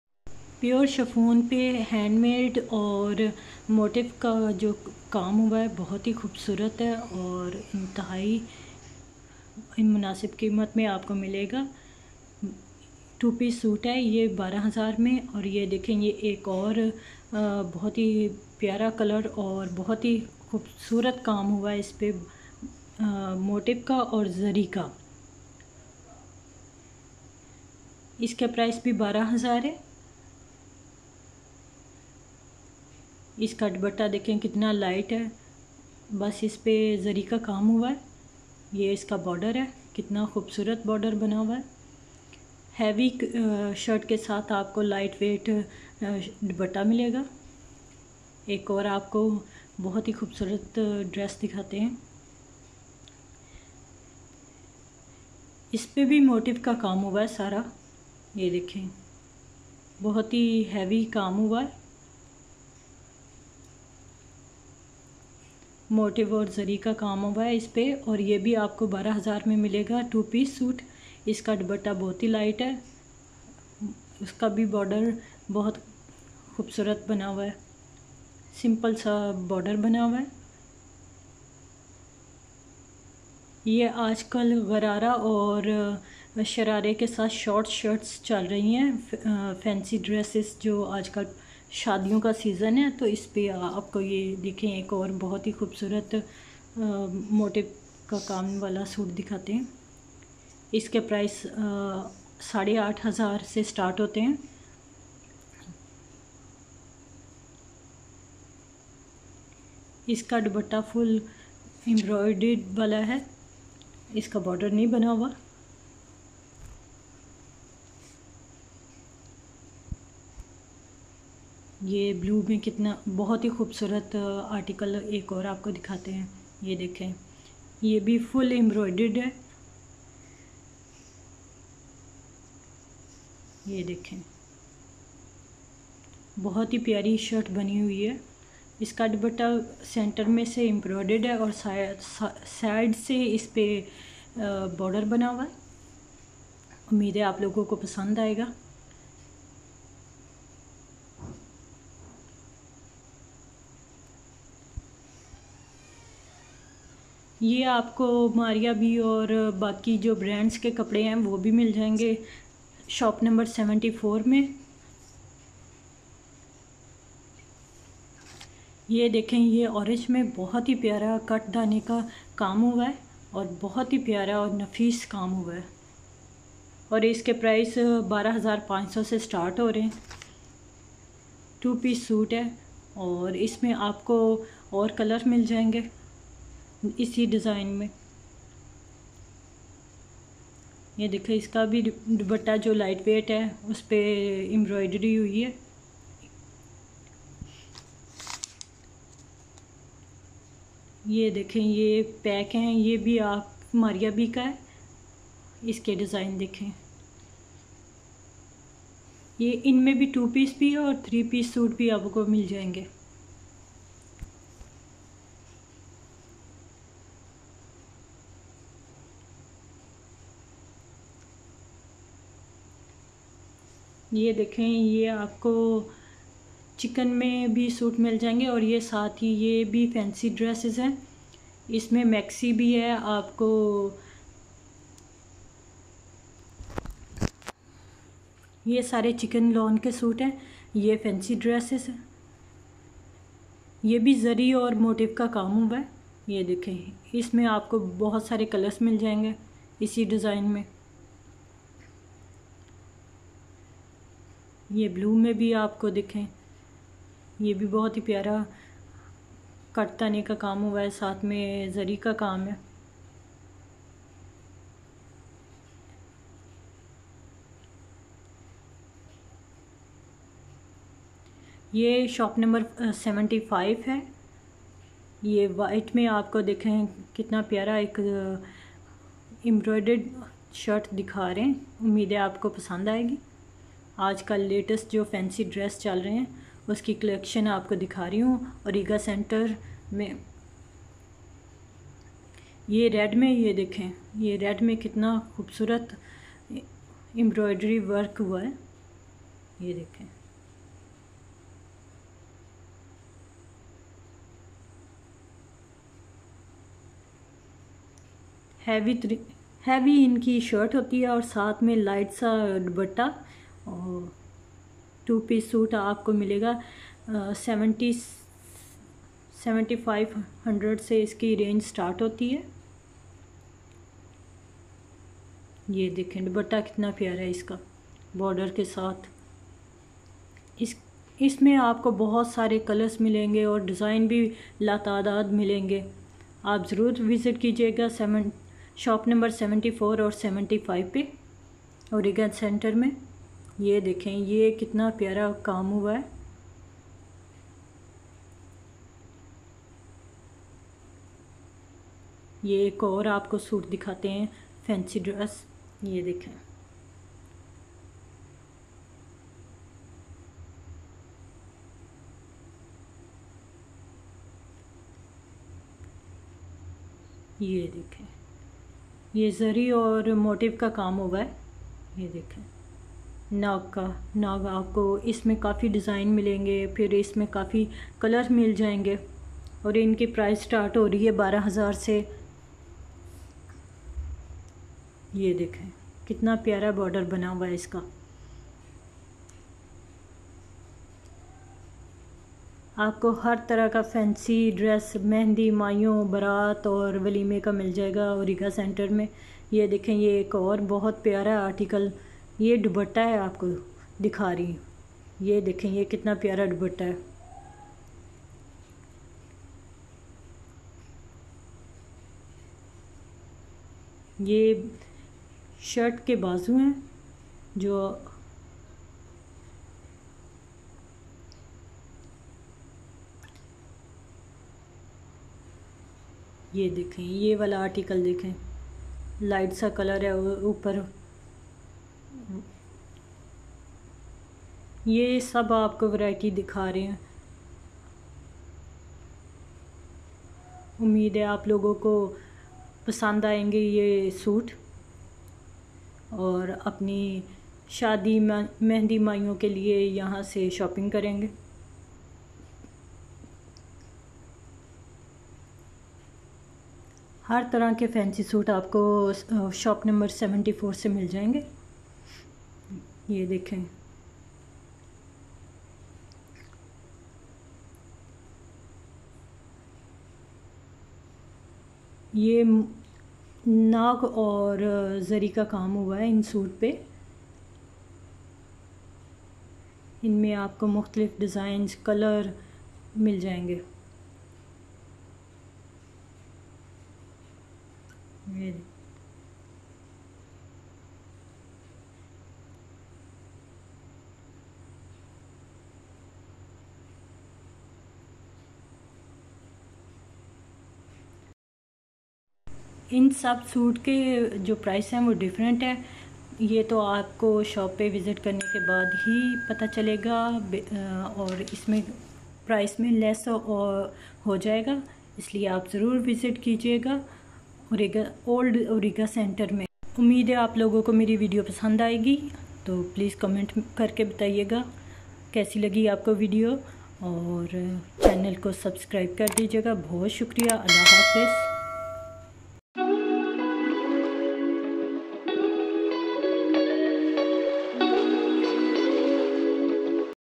Fancy suit collection in Old Auriga Market Lahore
A short shopping Vlog on OLD AURIGA MARKET LAHORE. Fancy suits much more expensive than the other ones. U can buy a cheap one and buy it for every function WEDDINGS, EID and other Functions.

Hope you ll enjoy shopping with me.